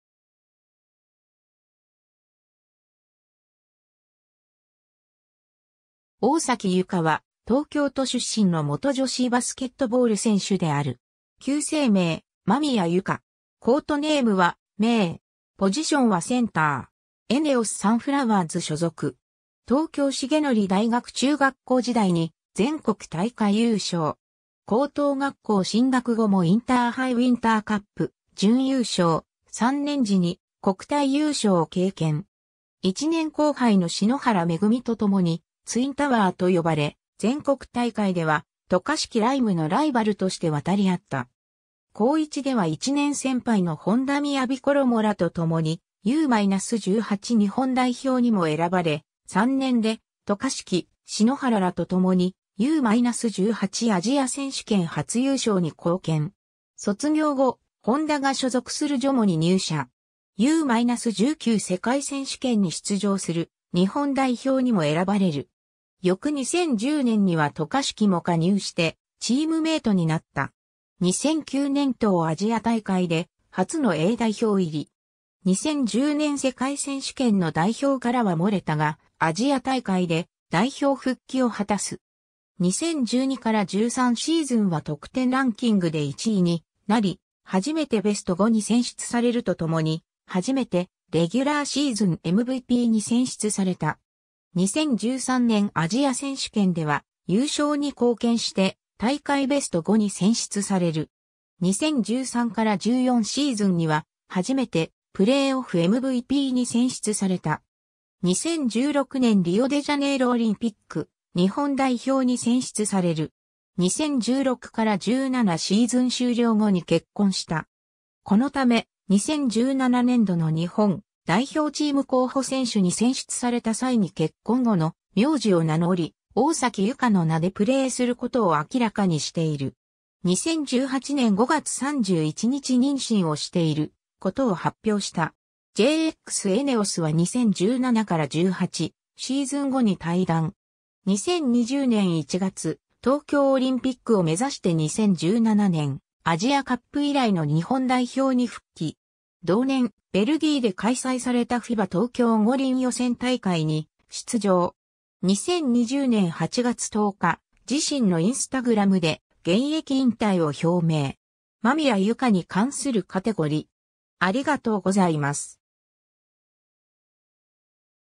大崎佑圭
大﨑 佑圭（おおさき ゆか、1990年4月3日 - ）は、東京都出身の元女子バスケットボール選手である。
旧姓名：間宮 佑圭（まみや・ゆか）。
コートネームは「メイ」。
ポジションはセンター。
ENEOSサンフラワーズ所属。
 東京成徳大学中学校時代に全国大会優勝。
高等学校進学後もインターハイ・ウィンターカップ準優勝、3年次に国体優勝を経験。
1年後輩の篠原恵とともに「ツインタワー」と呼ばれ、全国大会では渡嘉敷来夢（桜花学園）のライバルとして渡り合った。
 高1では1年先輩の本田雅衣らとともにU-18日本代表にも選ばれ、3年で渡嘉敷・篠原らとともにU-18アジア選手権初優勝に貢献。
 卒業後、本田が所属するJOMO（現エネオス）に入社。
U-19世界選手権に出場する日本代表にも選ばれる。
翌2010年には渡嘉敷も加入してチームメイトになった。
 2009年東アジア大会で初のA代表入り。
 2010年世界選手権の代表からは漏れたが、アジア大会で代表復帰を果たす。
 2012-13シーズンは得点ランキングで1位になり、初めてベスト5に選出されるとともに、初めてレギュラーシーズンMVPに選出された。
 2013年アジア選手権では優勝に貢献して大会ベスト5に選出される。
 2013-14シーズンには、初めてプレイオフMVPに選出された。
 2016年リオデジャネイロオリンピック日本代表に選出される。
 2016-17シーズン終了後に結婚した。
このため、2017年度の日本代表チーム候補選手に選出された際に結婚後の苗字を名乗り、大崎佑圭の名でプレーすることを明らかにしている。
 2018年5月31日妊娠をしていることを発表した。
JX-ENEOSは2017-18シーズン後に退団。
 2020年1月、東京オリンピックを目指して2017年アジアカップ以来の日本代表に復帰。
同年、ベルギーで開催されたFIBA東京五輪予選大会に出場。
 間宮佑圭に関するカテゴリ: